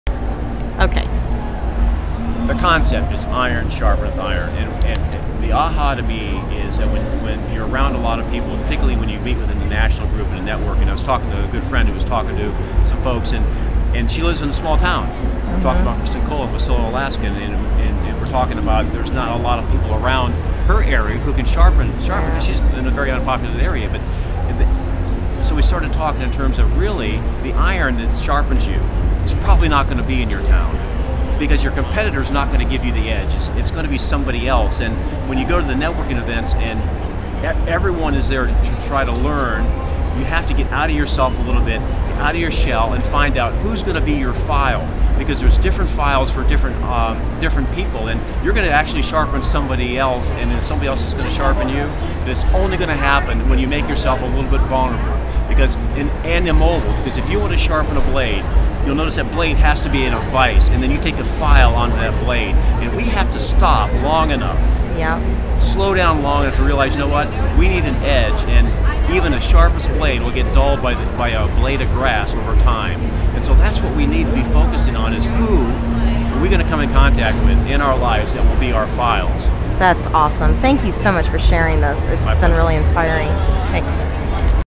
Real Estate Tribal Group
Hear about the upcoming Regional Tribal Gathering October 8-10 at the Omni Dallas Hotel, Dallas, TX